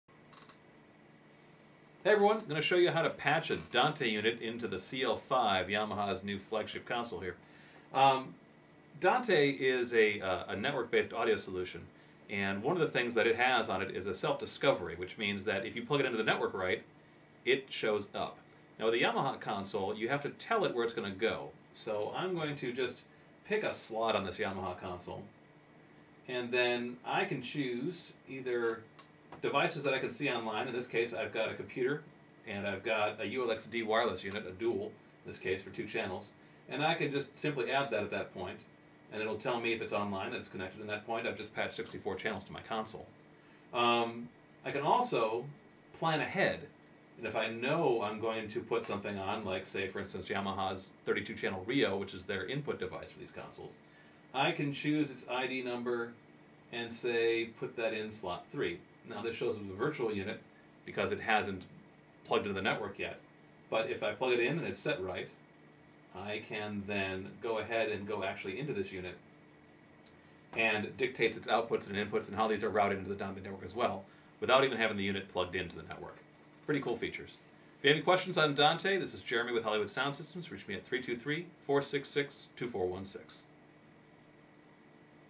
Yamaha CL5 - patching Dante
A brief tutorial about patching Dante on a Yamaha CL5.